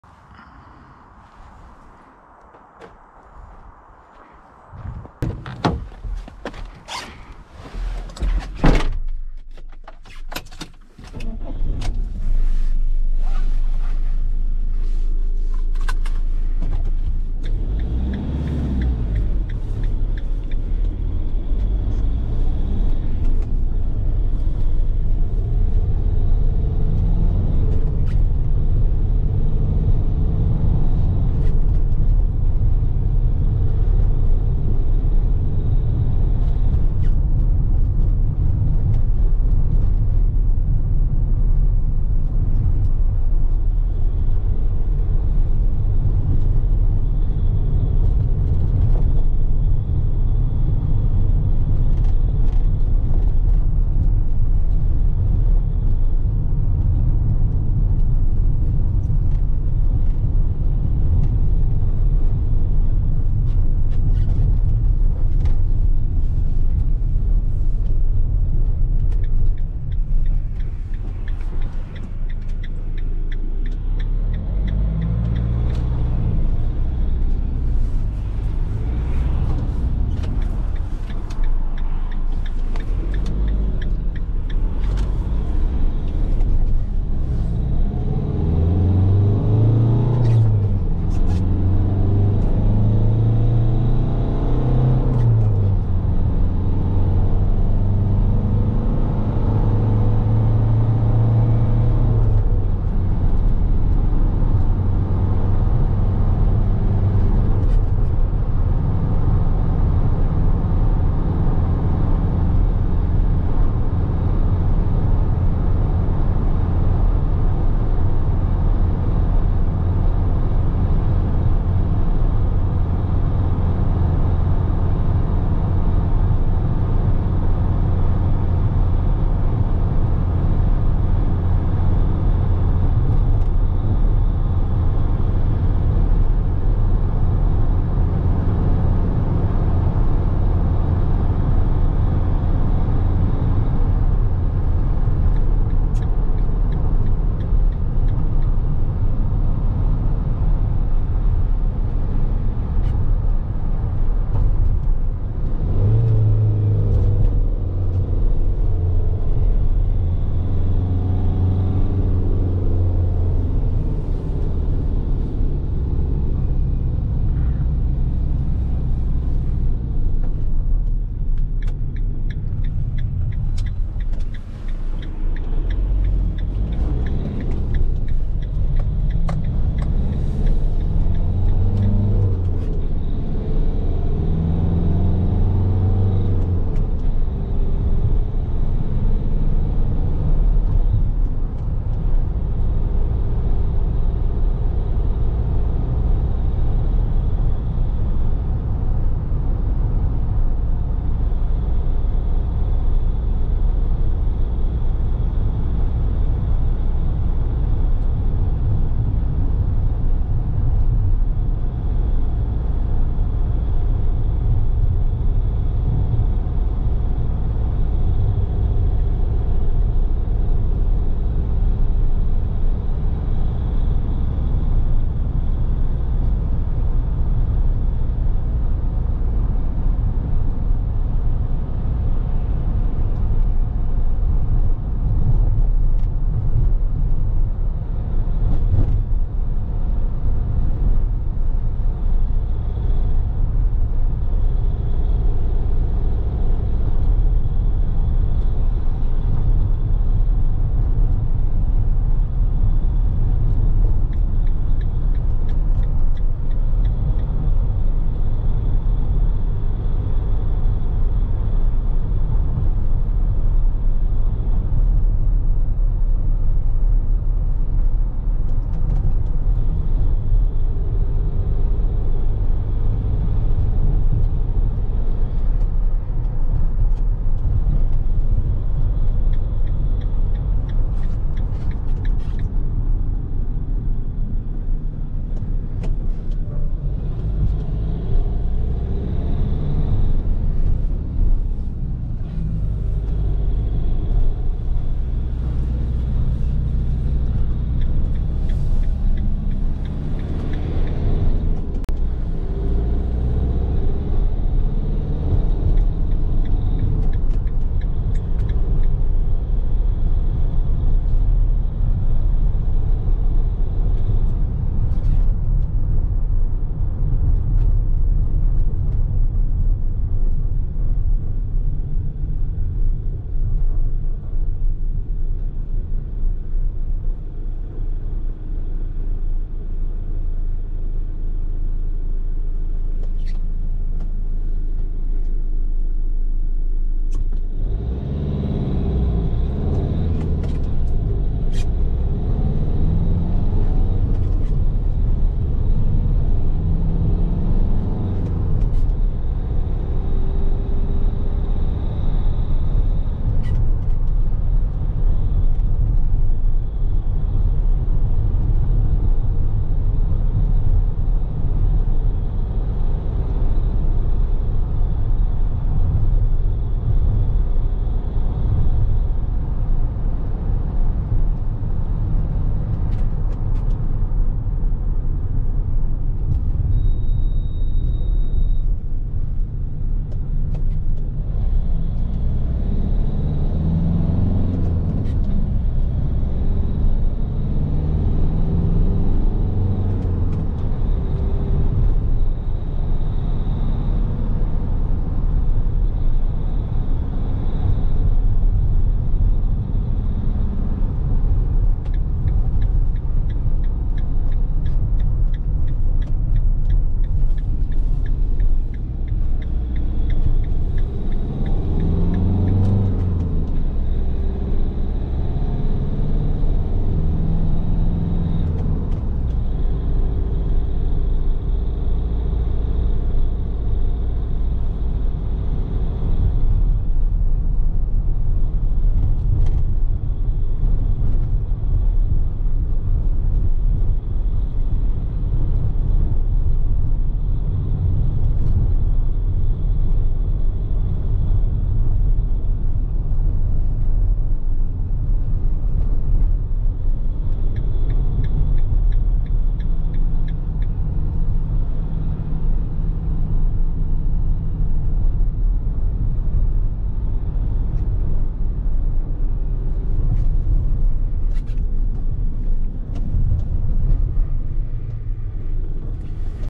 Fiat Ducato IV| 4K POV Test Drive #402 Joe Black
Car: Fiat Ducato IV

Engine: 2.3 MultiJet II Diesel
Power: 96 kW/130 HP
Torque: 220 NM
Transmission: 6 speed manual
Fuel Consumption by Fiat : 7,1 l /100 km
Fuel Consumption by Joe Black: 8,9 l /100km
0–100 km/h N/A
Top speed: 159 km/h
Weight: from N/A
Model: 2019

Driver: Joe Black
Video: GoPro Hero 8 Black
Edit: Final cut Pro X